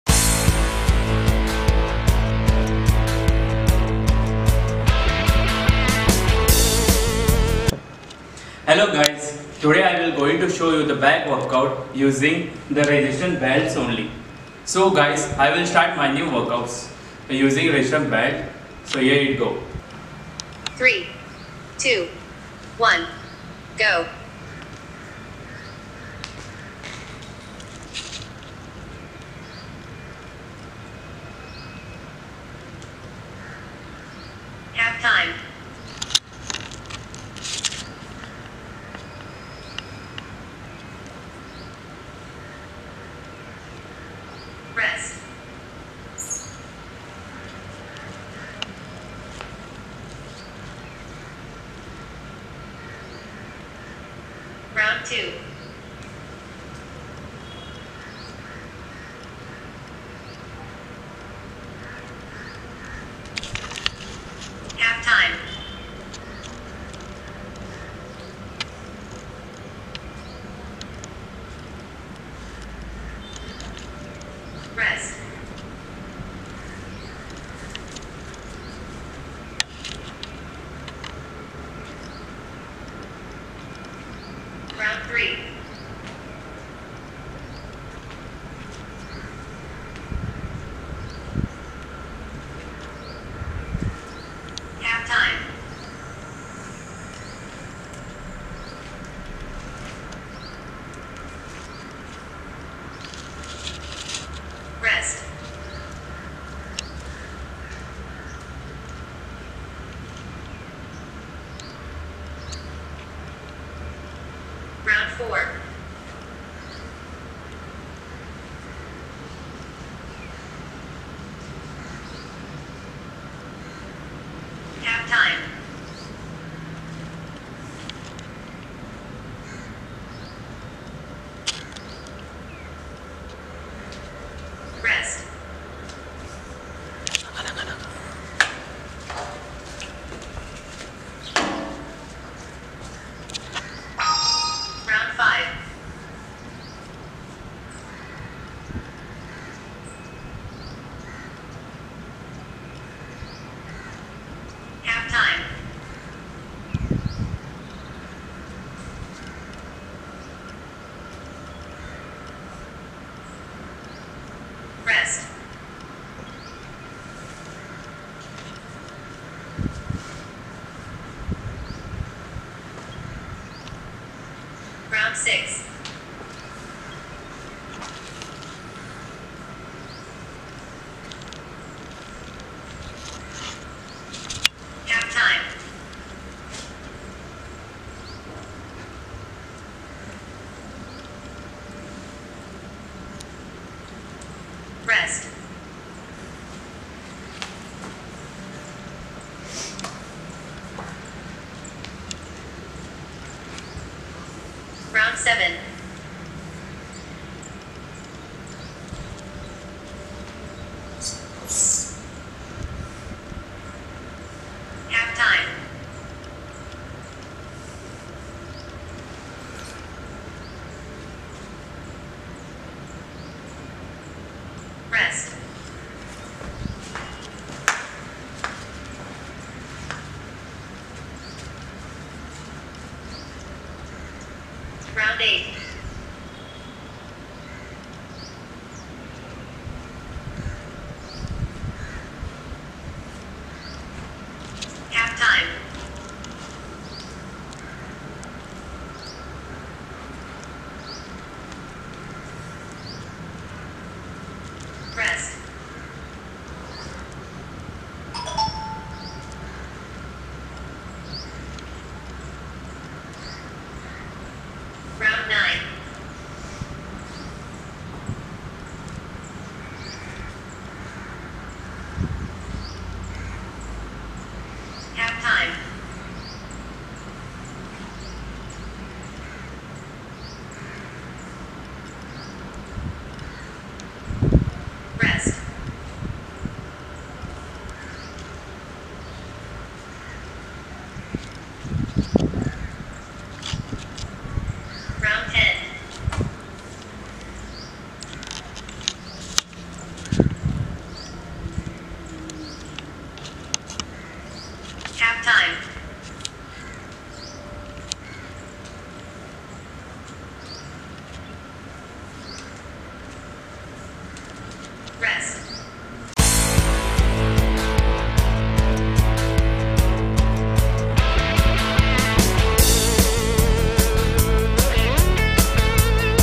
5 Minute Intense back workout using resistance band
Back workout using only resistance bands.
This workout is helpful when you are on a holiday so that you can continue with your normal schedule
Its a 5 minute only workout. complete the full round for 3 times.
One round equals one set so do 3 sets each
It is a follow along workout
You can use any level of resistance band depending on your workout experience.